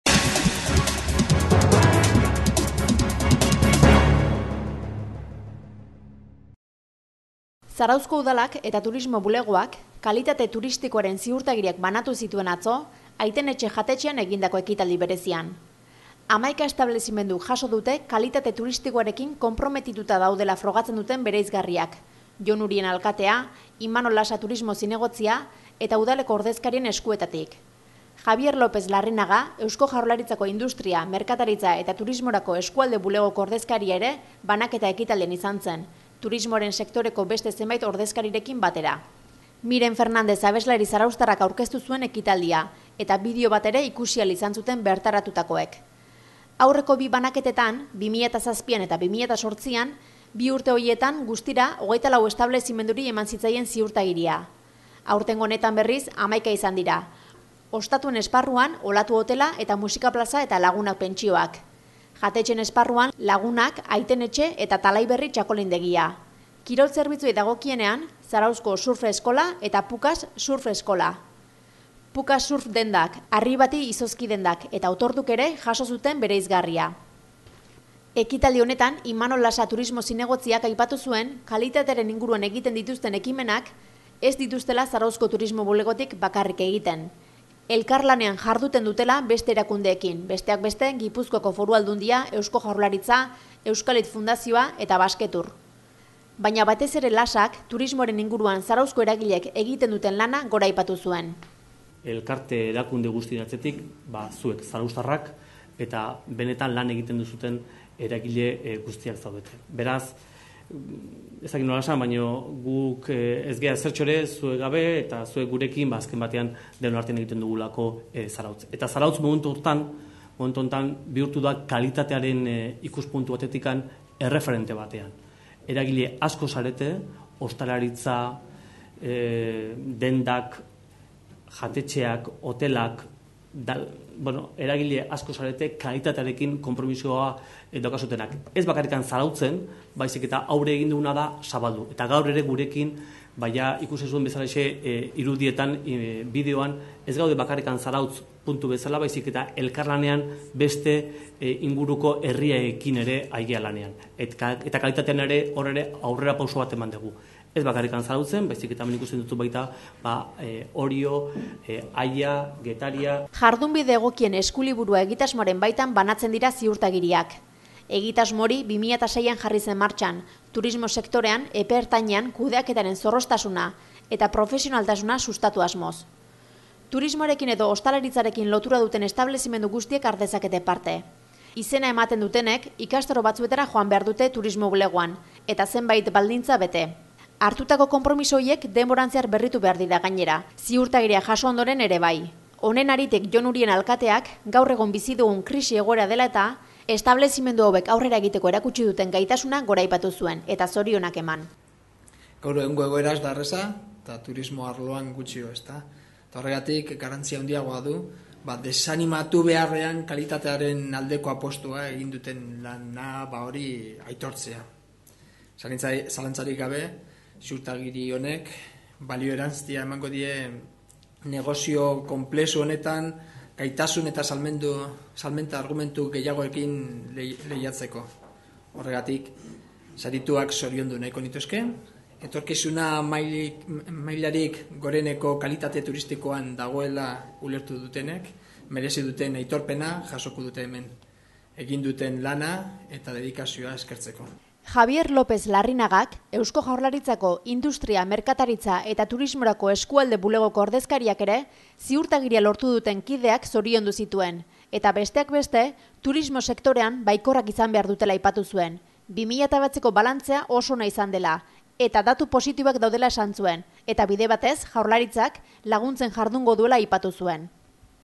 Zarautz Kalitate Turistikoarekin konprometitua.
Zarauzko  hamaika establezimenduk Kalitate Turistikoarekiko Konpromisoaren Ziurtagiria jaso zuten, 2010ko maiatzaren 25ean.